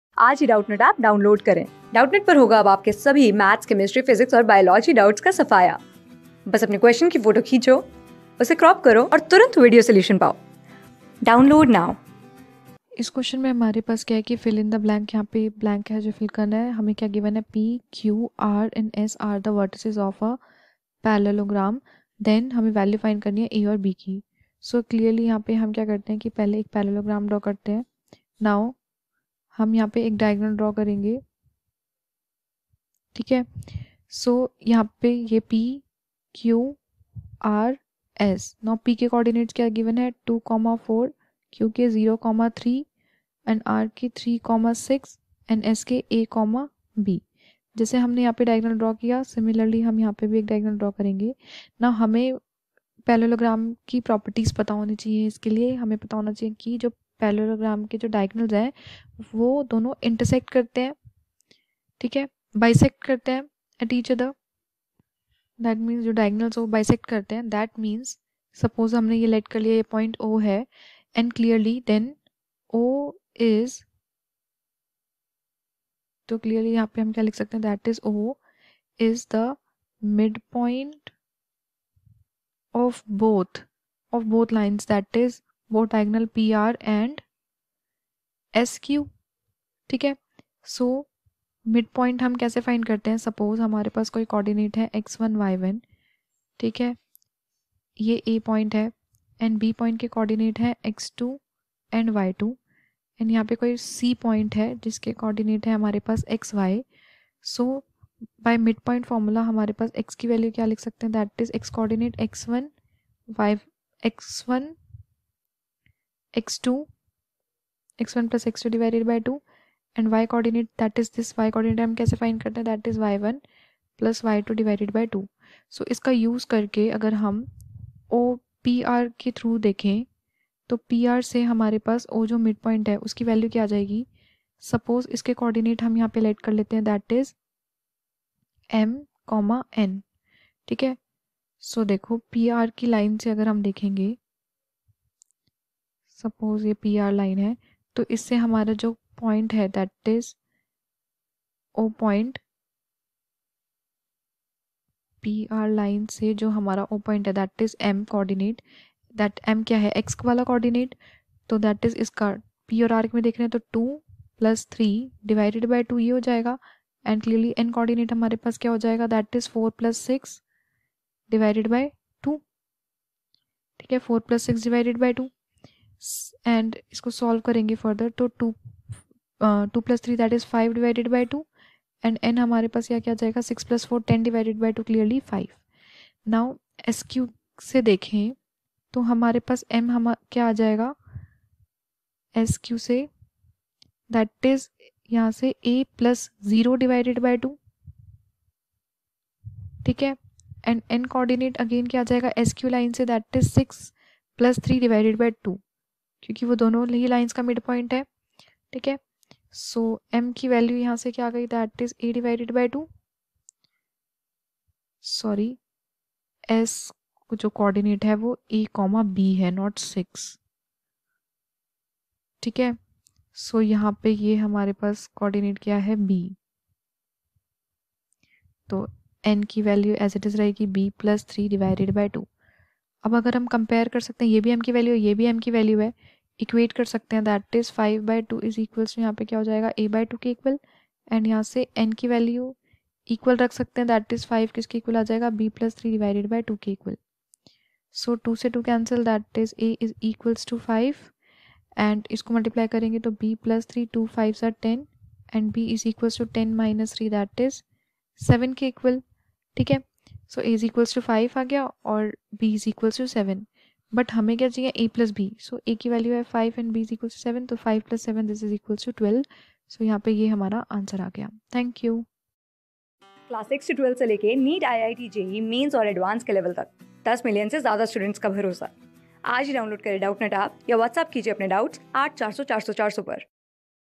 Fill in the blanks:If P(2, 4), Q(0, 3), R (3, 6) and S(a, b) are vertices of a parallelogram the...
Fill in the blanks:If P(2, 4), Q(0, 3), R (3, 6) and S(a, b) are vertices of a parallelogram then the value of a + b is
Class: 10
Subject: MATHS
Chapter: PRACTICE PAPER II
Board:CBSE

👉 Have Any Query? Ask Us.